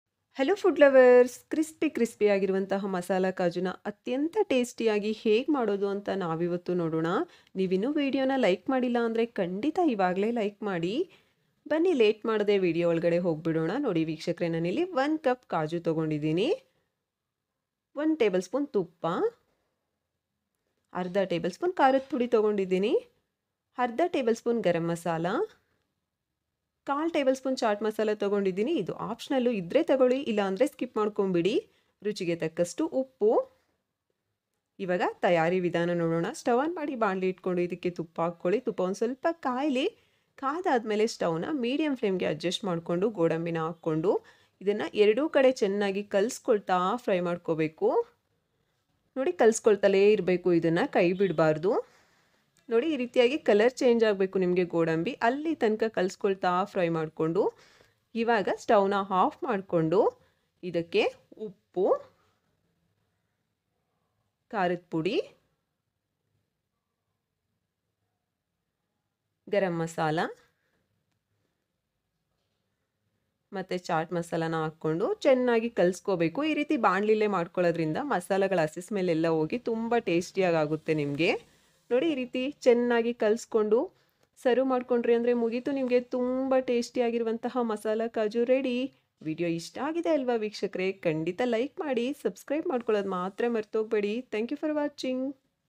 Masala cashew nuts recipe in kannada | ಮಸಾಲಾ ಗೋಡಂಬಿ | Masala kaju recipe | Masala godambi
About the video:

This video helps in making super delicious  masala cashew nuts \ masala kaju nuts \ roasted cashew nuts using a few ingredients.

ಈ ವಿಡಿಯೋ ನಮಗೆ ಮಸಾಲ ಗೋಡಂಬಿ ಅತ್ಯಂತ ರುಚಿ ಯಾಗಿ ಸುಲಭ ವಿಧಾನದಲ್ಲಿ ಮಾಡಲು ಸಹಾಯ ಮಾಡುತ್ತದೆ.